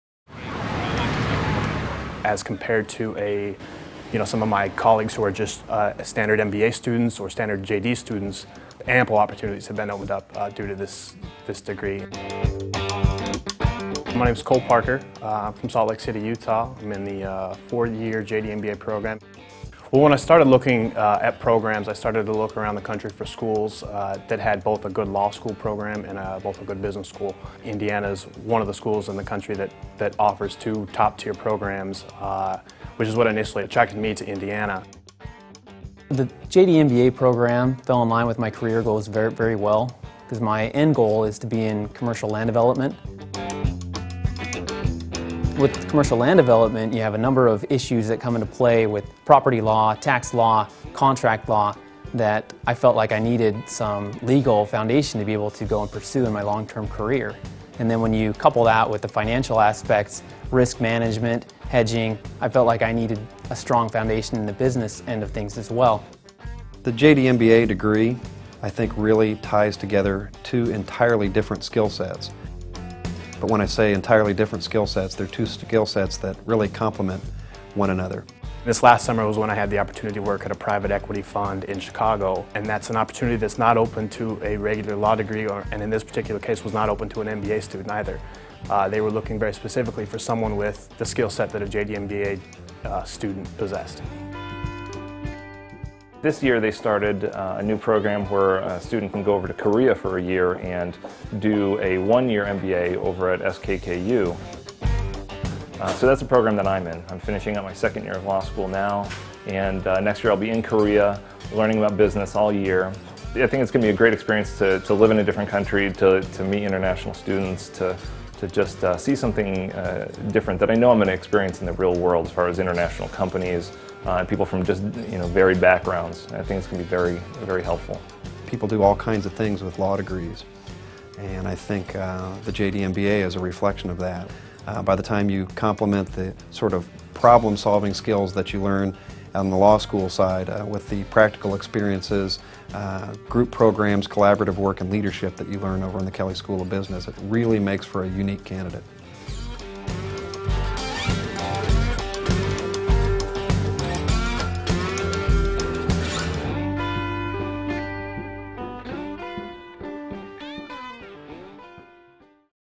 JD/MBA Joint Degree Program
The JD/MBA program leverages the talent and reputation of IU's top-rated Kelley School of Business. Hear what our students say about it.